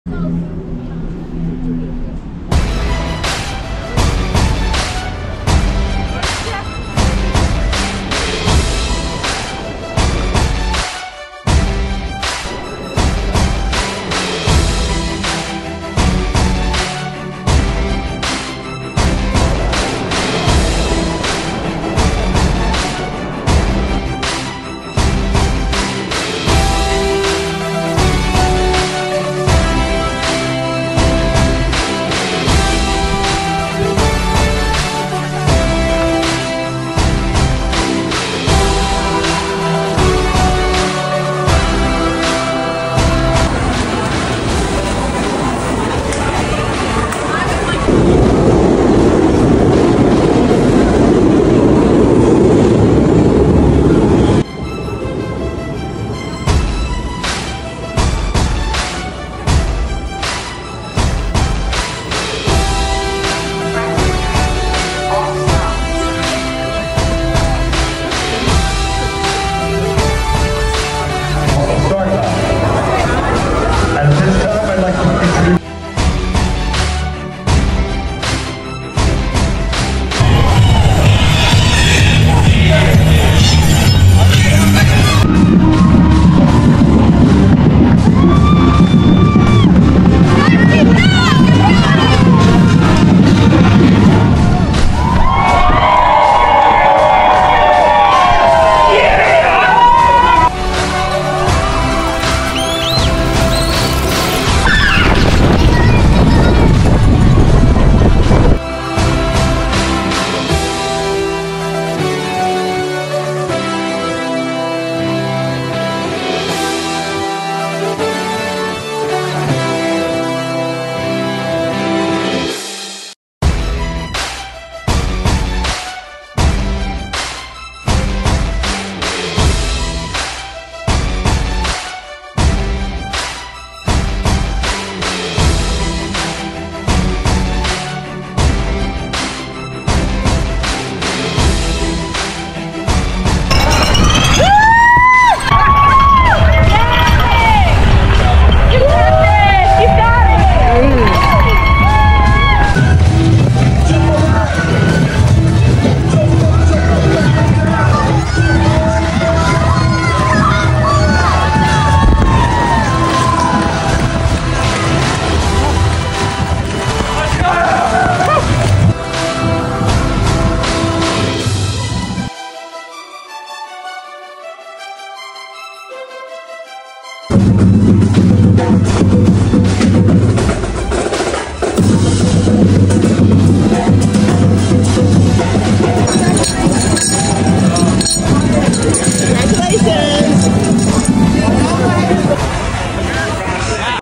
2023 Broad St Run!
2023 Philadelphia IBX Broad Street run! I aopologize for the shaky footage. I forgot to turn on stabilization on my gopro like an idoit 🥲🤦🏻‍♂️